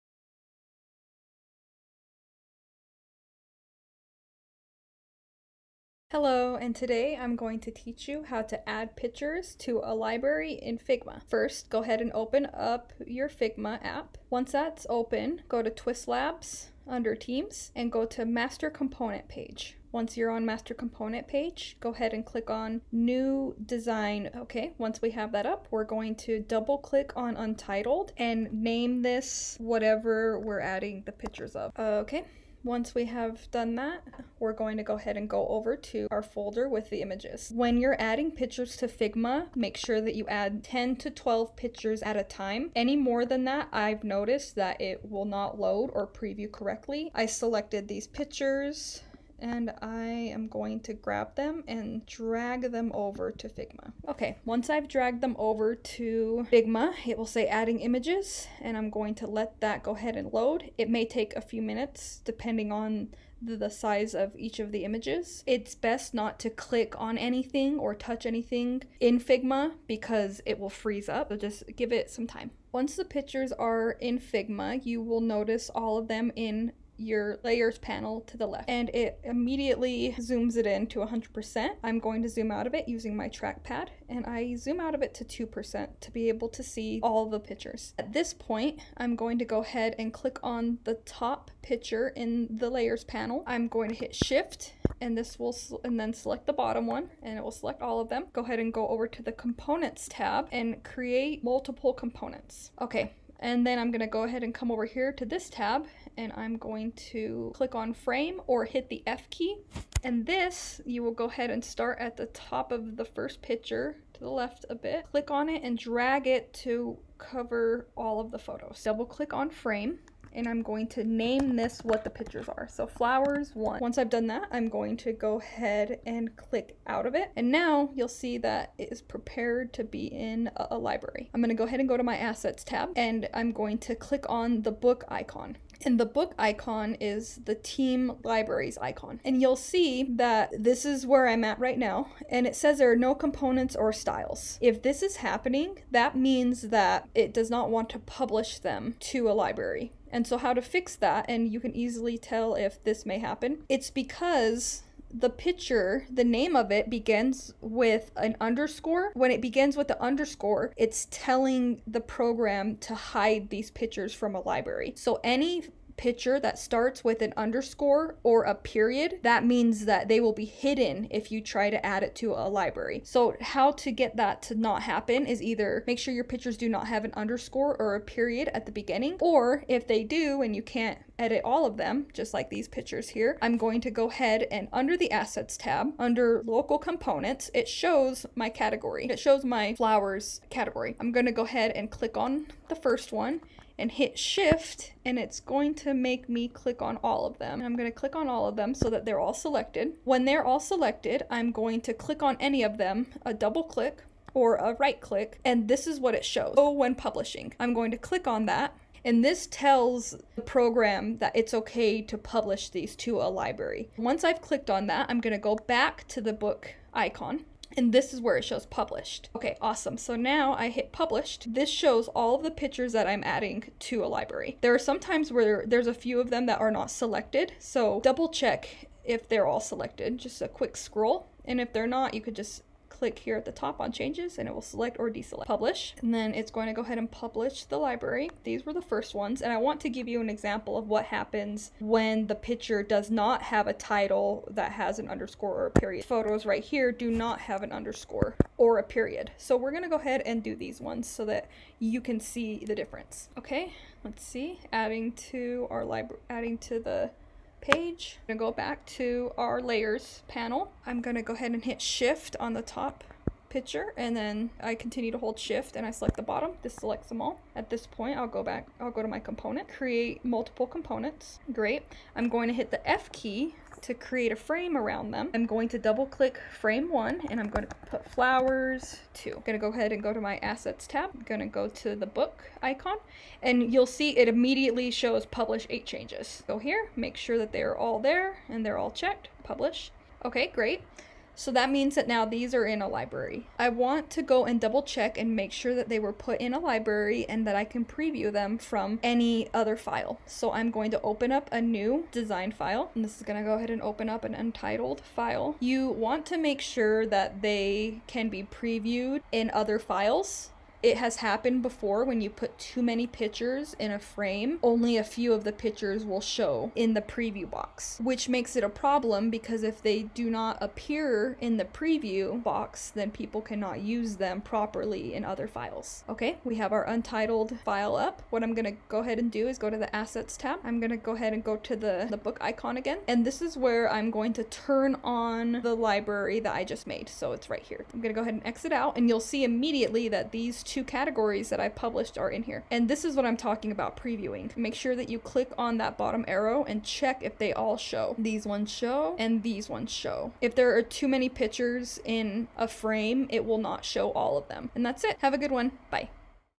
Adding photos to a library in Figma.
In this video, I will cover how to add photos to a Figma library.

Using Figma’s library option allows your team or clients to easily access stock photos for any design.

Figma supports images in the following formats: PNG, JPEG, TIFF, GIF, or WEBP.

*Top takeaways*

Do not upload more than ten photos at a time to a Figma library.

If the name of the photos has “_” or. “.” it’s telling Figma to hide these photos from being published in a library. Before publishing, go to the asset tab, and under local components will show the pictures on the page. Select them all and right-click on any photo; an option will appear that says “Show when publishing” click on this option. Now you can publish the pictures in the library.

Be sure to check in a new file that all your uploaded photos are previewing correctly.